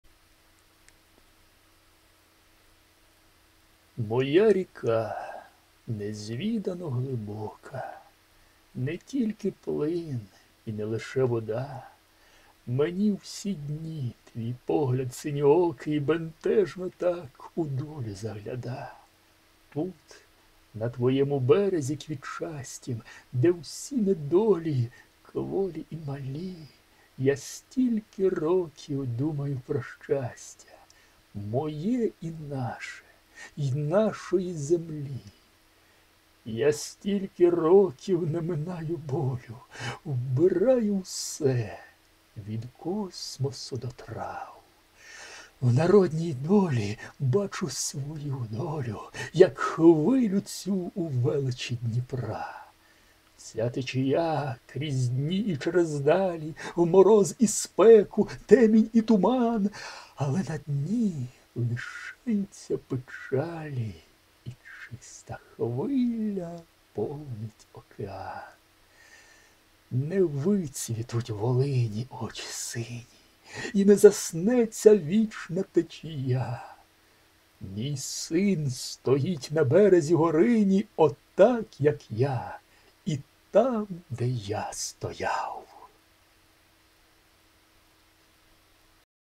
Василь Басараба - Горинь
Читає Василь Білоцерківський.